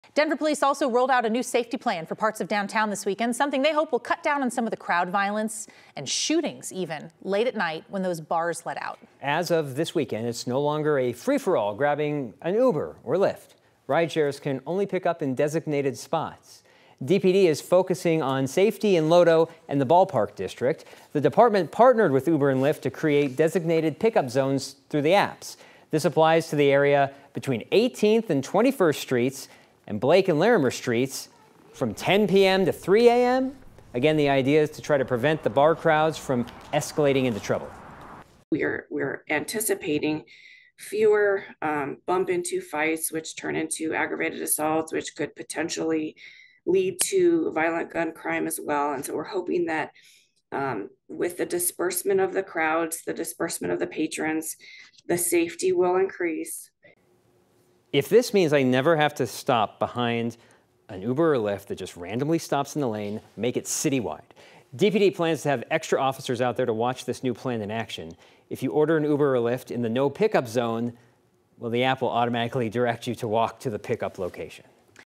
Denver Police roll out new safety plan for rideshares in downtown
Denver Police rolled out a new safety plan for parts of downtown this week by having rideshares only pick up in designated spots.

9NEWS (KUSA) is located in Denver, Colorado.